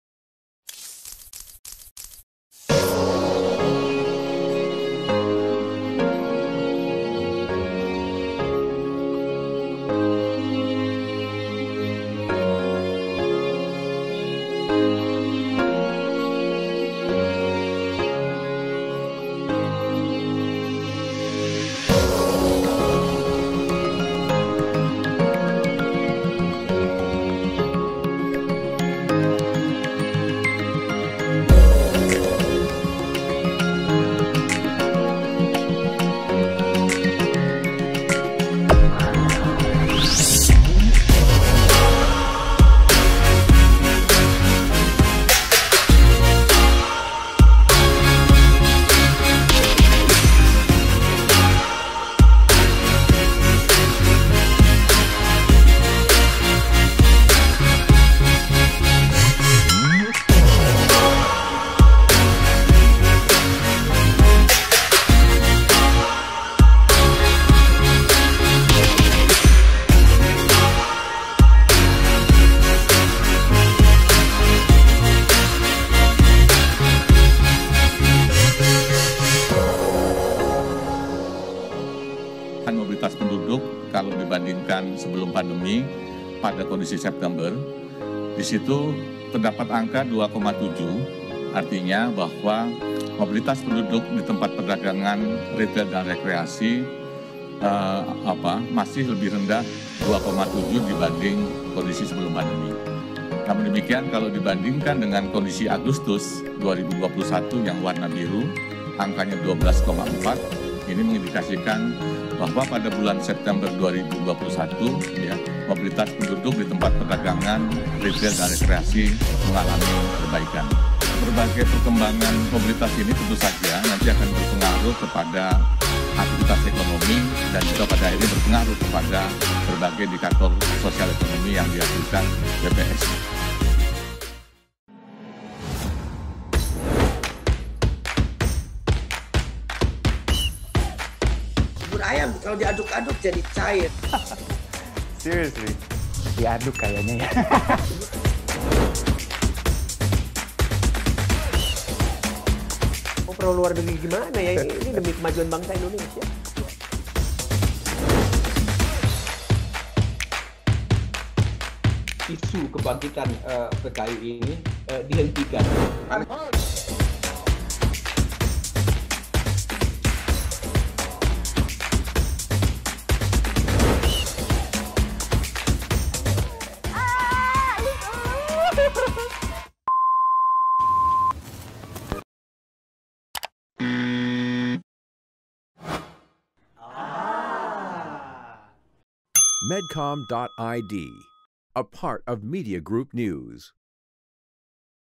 Mengalami Perbaikan Mobilitas, PPKM Berakhir Hari Ini
Badan Pusat Statistik (BPS) mencatat sepanjang bulan September lalu atau sejak pelonggaran aktivitas terjadi peningkatan di beberapa tempat mobilitas masyarakat dibandingkan bulan agustus. Berdasarkan data olahan dari Google Mobility, Badan Pusat Statistik (BPS) Mobilitas di tempat belanja dan kebutuhan sehari-hari meningkat menjadi 20,2% di bulan September dari sebelumnya 15,5% di bulan Agustus. Selain itu mobilitas masyarakat di tempat perdagangan ritel dan rekreasi jumlahnya meningkat dari 12,4% pada bulan agustus menjadi -2,7% pada bulan septemper. Begitu juga mobilitas di tempat kerja per September -16,6% atau lebih baik dari bulan agustus yang mencapai 22,6%. Kepala BPS, Margo Yuwono mengatakan meski beberapa mobilitas masyarakat masih minus namun jumlahnya lebih rendah sehingga menunjukan perbaikan mobilitas.